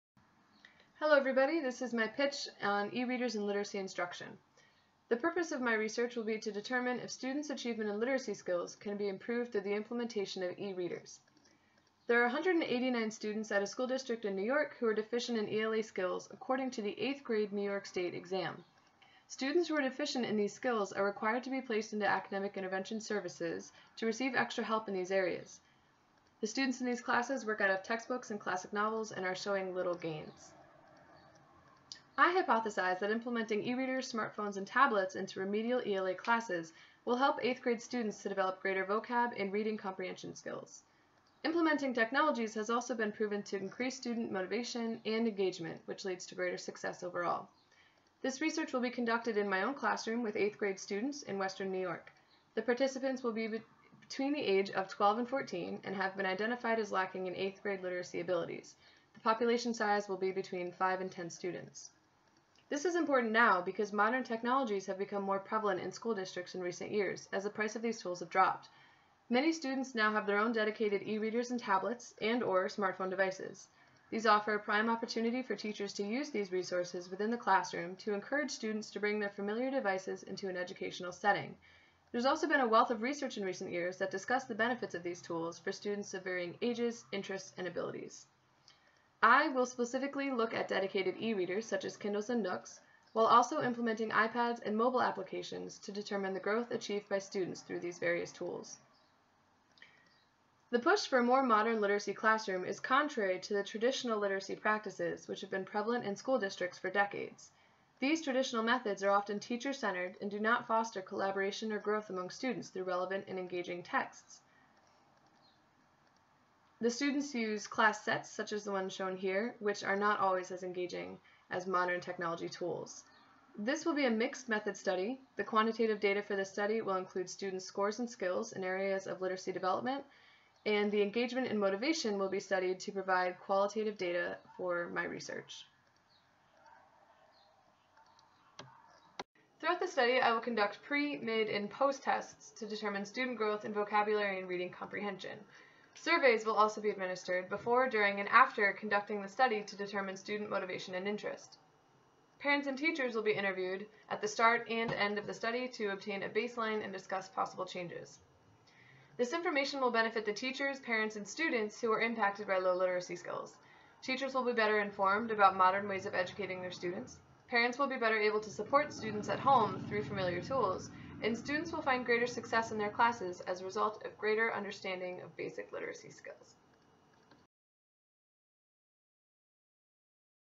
Research Pitch
A brief description of my research plan for my Educational Technology Graduate program at SUNY Oneonta.